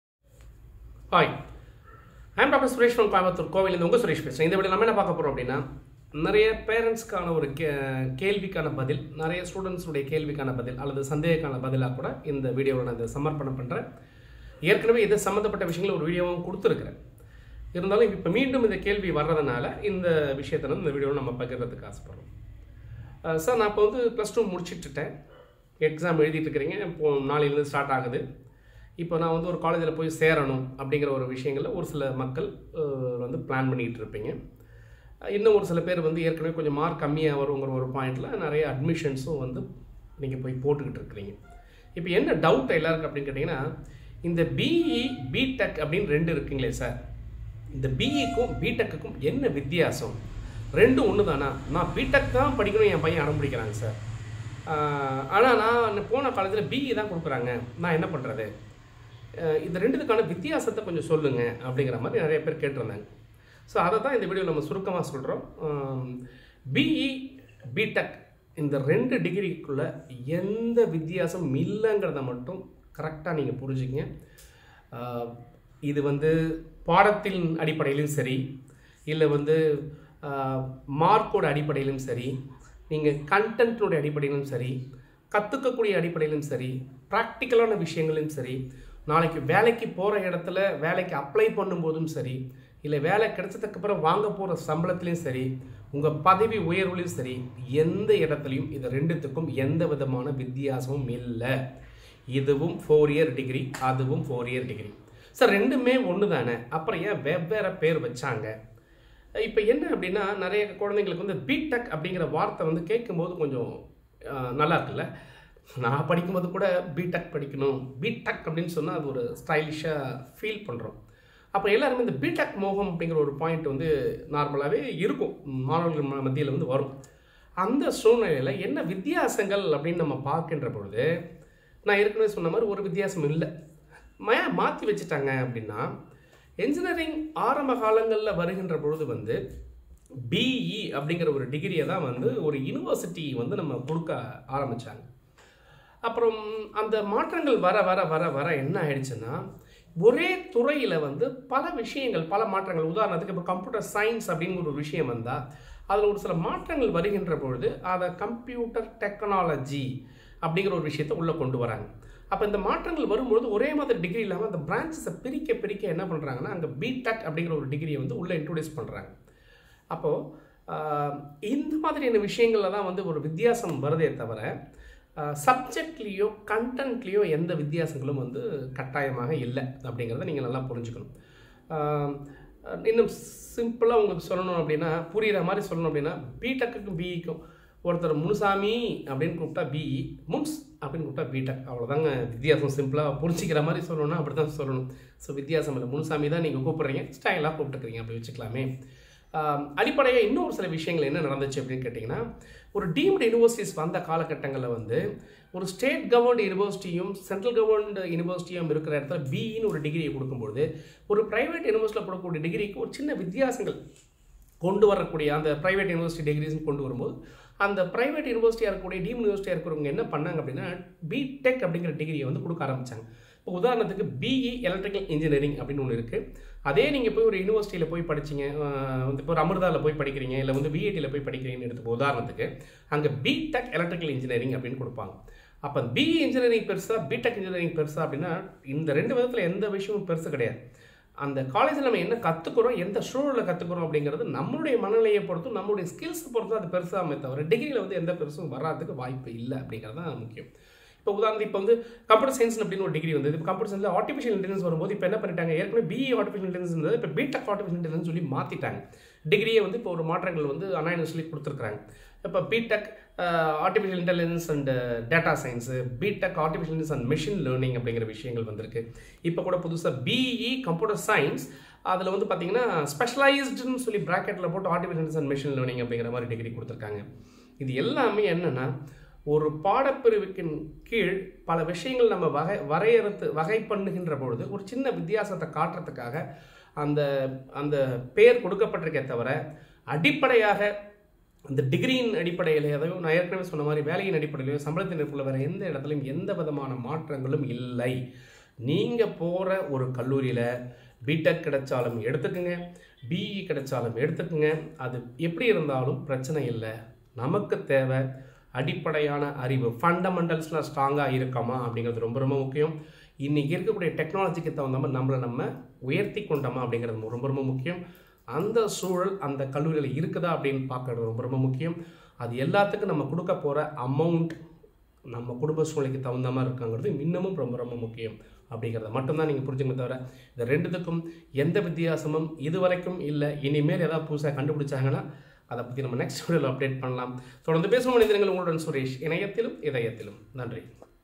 B.E/B.Tech Which is best?|வித்தியாசம் என்ன?எது சிறந்தது எதை படிக்கலாம்|College/Branch ah?|குழப்பமா?
B.E/B.Tech Which is best?|எது சிறந்தது எதை படிக்கலாம்|College/Branch ah?|எது முக்கியம்|Full details|Suresh Seetharaman